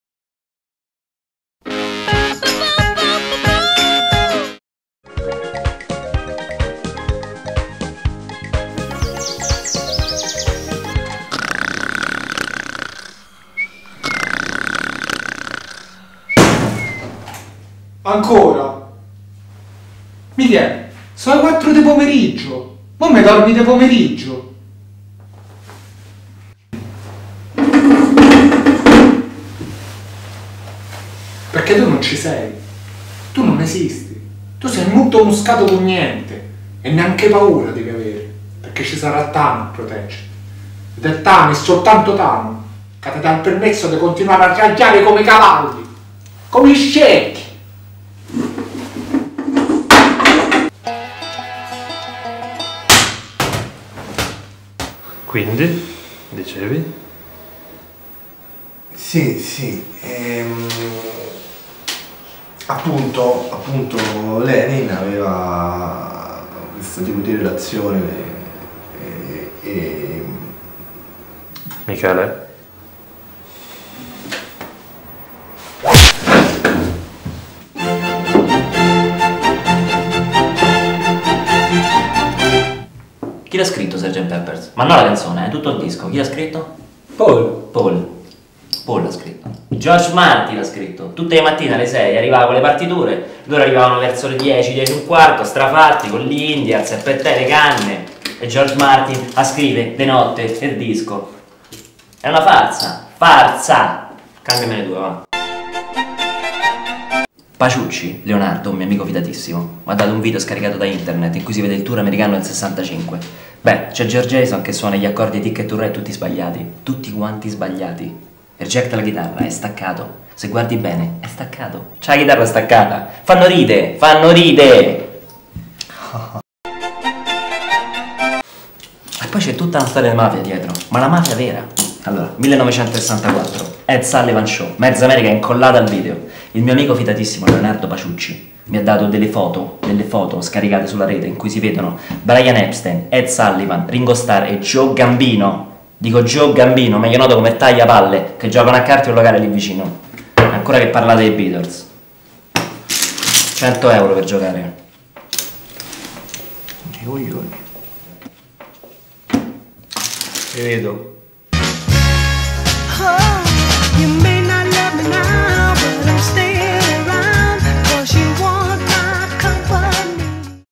Io Brancolo Episodio 3
Tante frustrazione di un giovane nel preparare l'esame di Relazioni Internazionali, di Michele m Bertini & Enrico De Gasperis